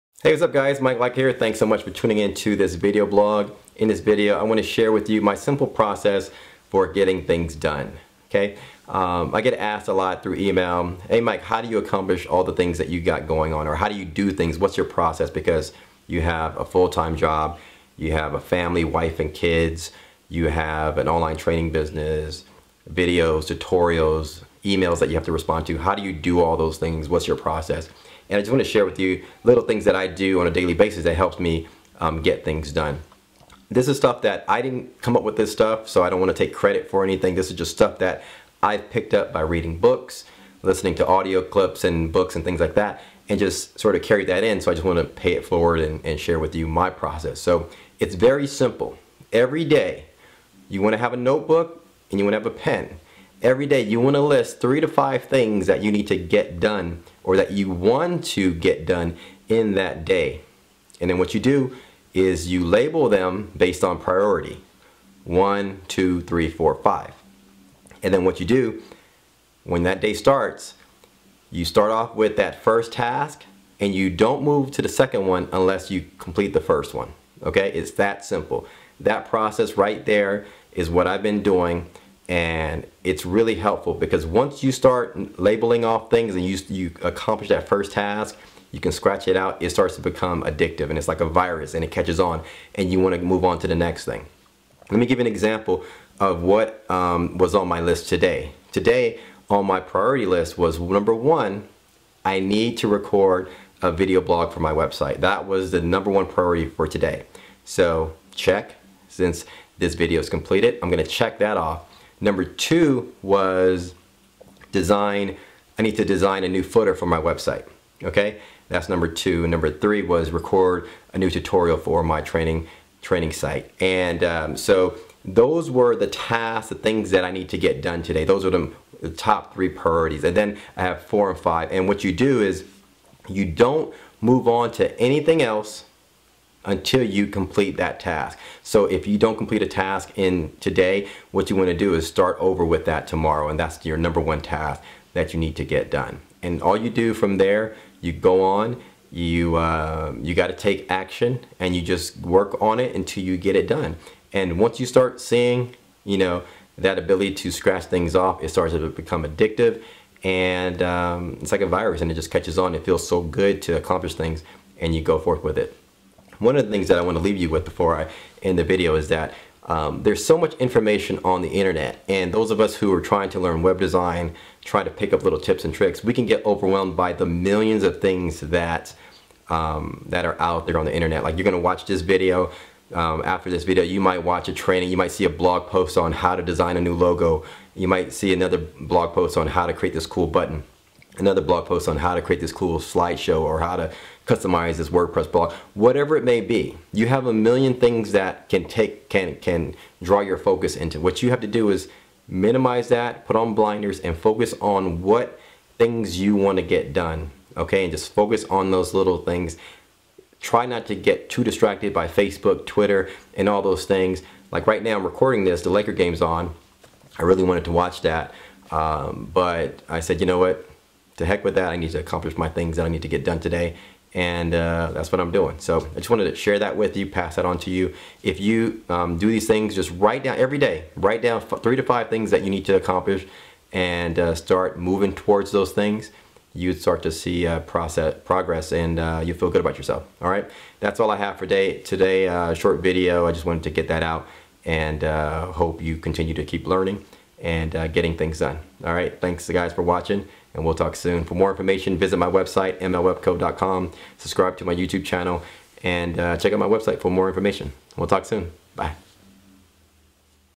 My Simple Process for Getting Things Done
- In this video I share my simple process for getting things done. Enjoy!